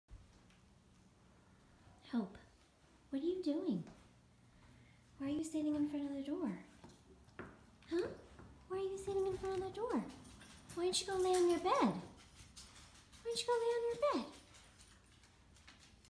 Hope - Relaxing at Home (finally)
After several days in the ICU, Hope prefers the hardwood floor over her giant, comfy bed.